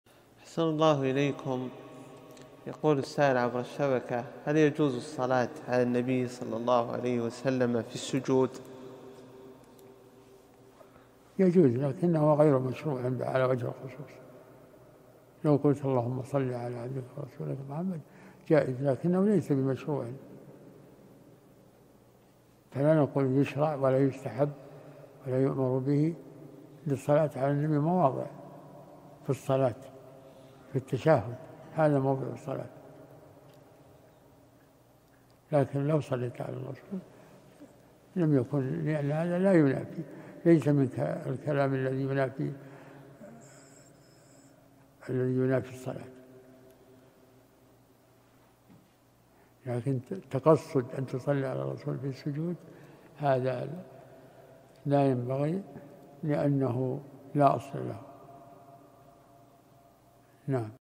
Secdede Salâtüselâm Okunur mu?
Whatsapp haber listemize katılmak için: Bu linke tıklayıp mesaj atınız.‎

‎ Şeyh Abdurrahmân b. Nâsır el-Berrak'a soruldu: Secdede Nebî sallallahu aleyhi ve sellem'e salât etmek câiz midir?

Cevâb: Câiz, ancak husûsî bir şekilde belirlenmiş (meşrû) değildir. Eğer "Allâh'ım kulun ve rasûlun olan Muhammed'e salât et" dersen câizdir. Lâkin bu, belirlenmiş (meşrû) değildir. Meşrûdur, müstehabdır veya emrolunmuştur, demeyiz. Nebî sallallahu aleyhi ve sellem'e salâtüselâm etmek için bazı yerler vardır. Namazda teşehhütteki salât buna örnektir; bu, salâtüselâmın namazdaki yeridir. Ancak (secdede) rasûle salât etmende bir sıkıntı yoktur. Çünkü bu namaza aykırı olan konuşma cinsinden değildir. Ancak rasûle secdede salat etmeyi özellikle kasdetmen uygun değildir. Çünkü bunun bir aslı yoktur.